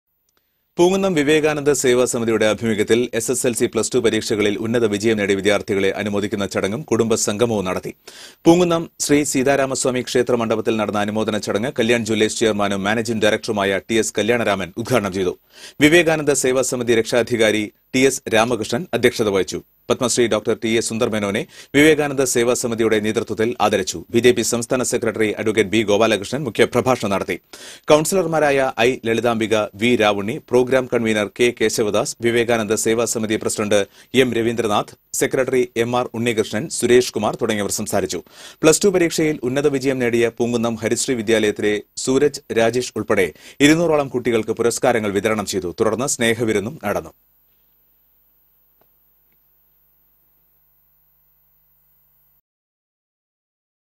Poonkunnam Vivekananda Seva Samithi
തൃശൂരിലെ പ്രമുഖ വ്യവസായി സുന്ദര്‍ മേനോൻ , പൂങ്കുന്നം വിവേകാനന്ദ സേവാ സമിതിയുടെ ആഭിമുഖ്യത്തിൽ നടന്ന ചടങ്ങിൽ  10 , 12 ക്‌ളാസ്സുകളിൽ ഉയർന്ന മാർക്ക് നേടിയ കുട്ടികളെ അഭിനന്ദിക്കുന്നു